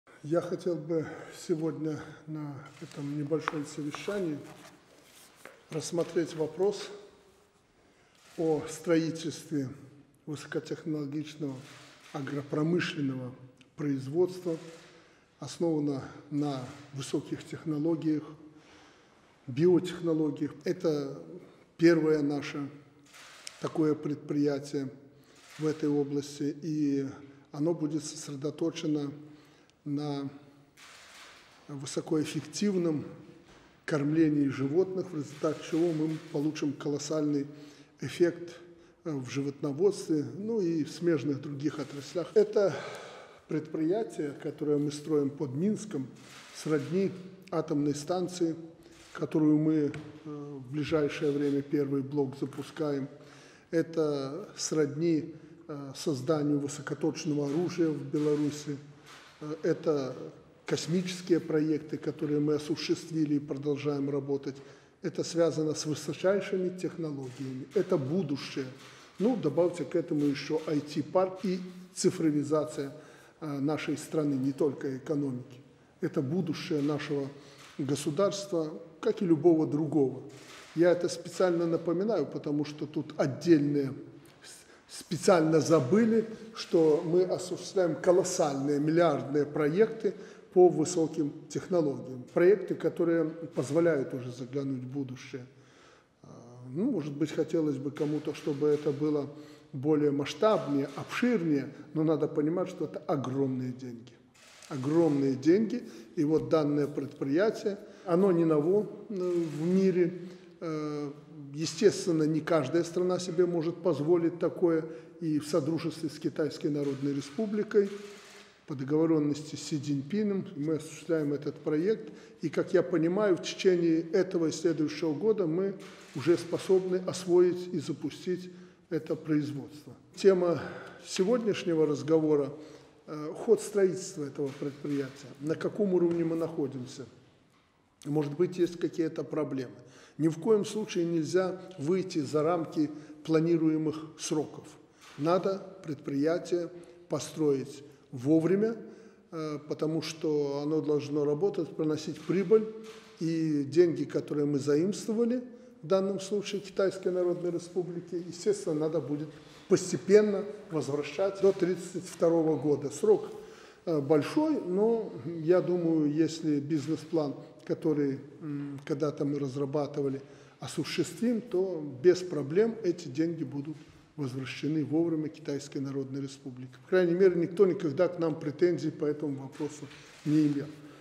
У Лукашенко обсудили ход строительства под Минском высокотехнологичного агропроизводства
Президент Беларуси Александр Лукашенко провел сегодня совещание, на котором обсуждалась реализация проекта по организации под Минском высокотехнологичного агропромышленного производства полного цикла.